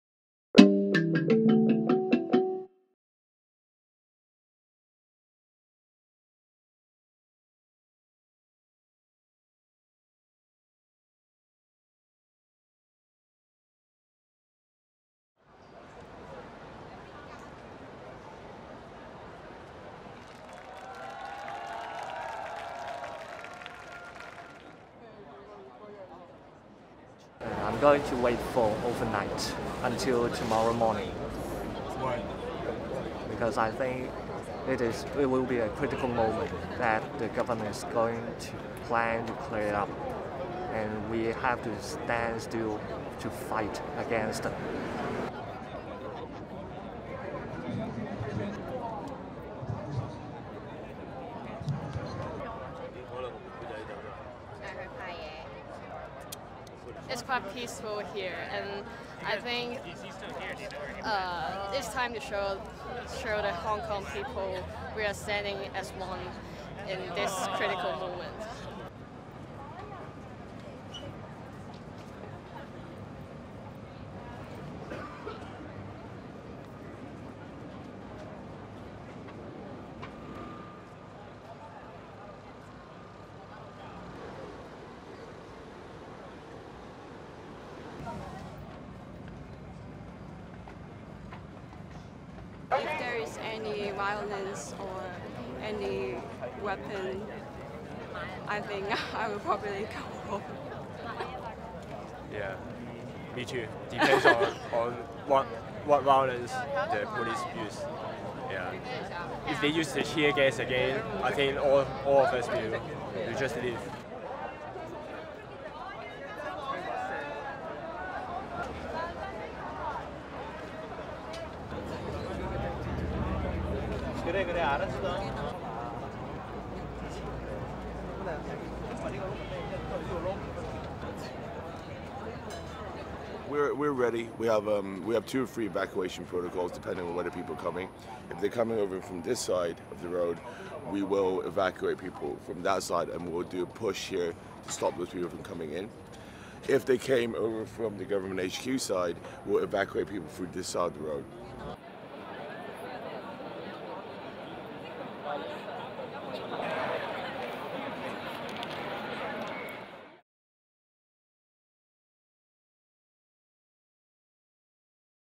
Hong Kong Protests Continue as Government Deadline Nears
Here are sounds and images from Day 9 of the student-led ‘Umbrella Revolution.’